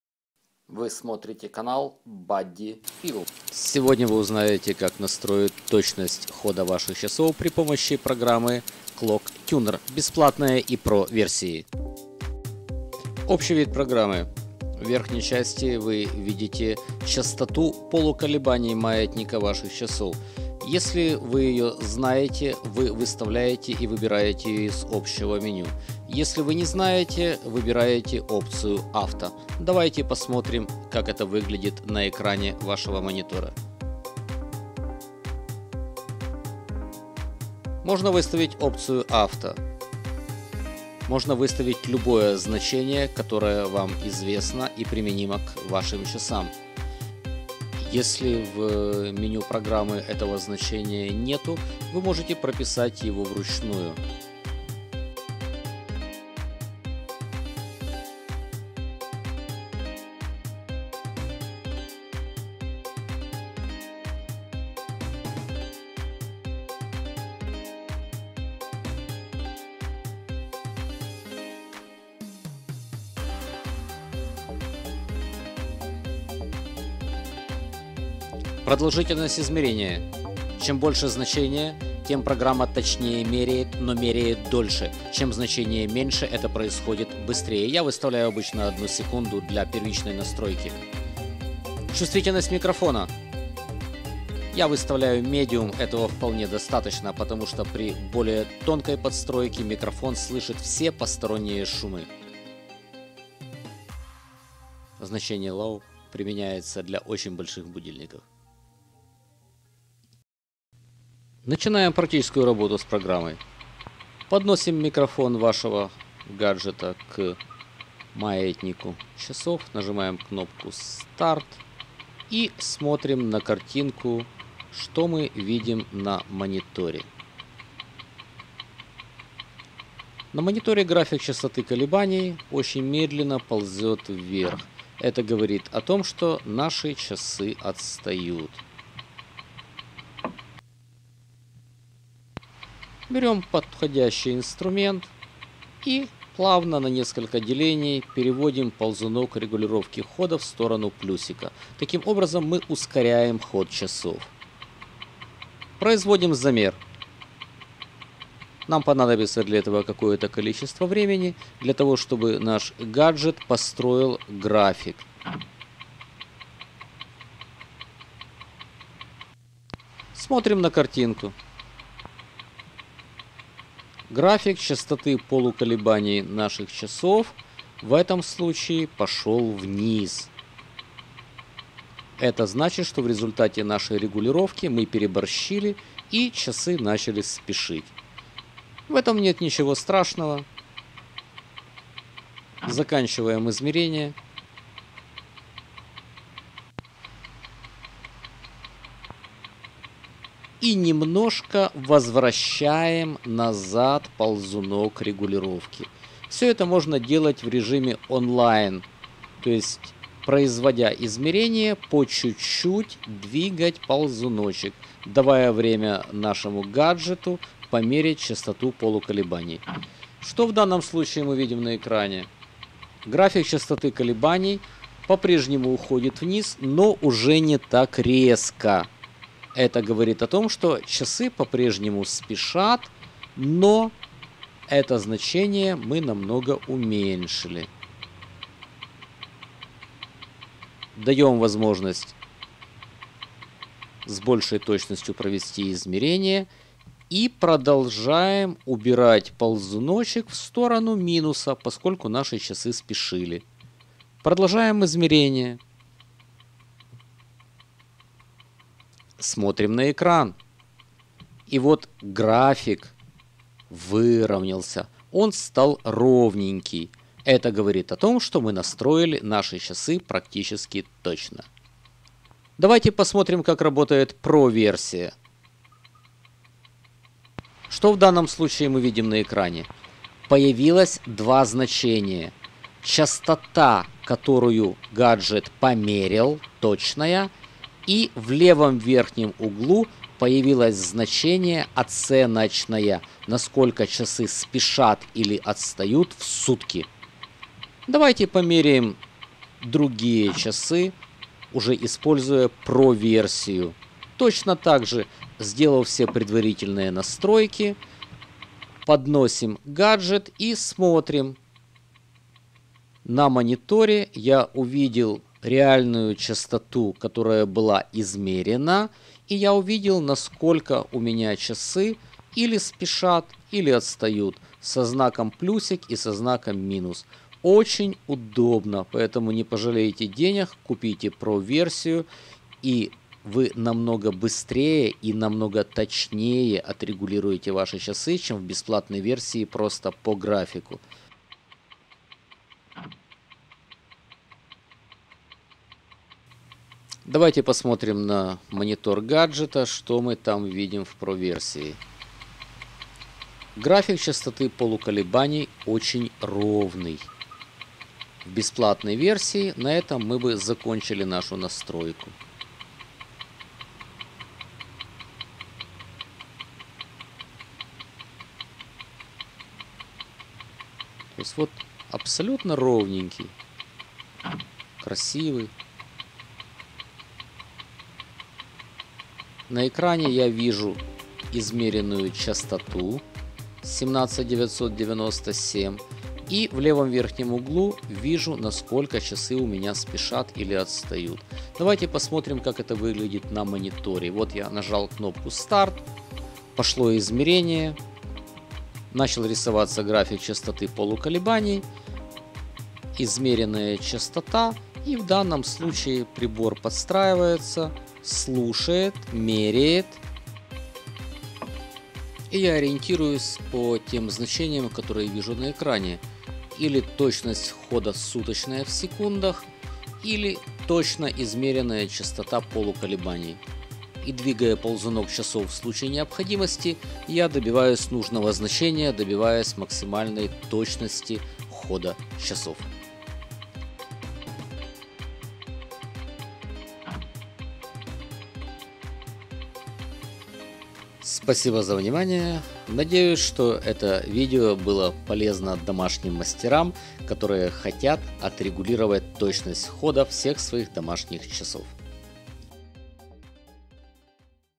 Проверка регулировка точности хода часов \ Андроид \ Как самому настроить часы \ Clock Tuner
Полная инструкция по использованию программы Clock Tuner  на Android для регулировки и проверки точности хода механических часов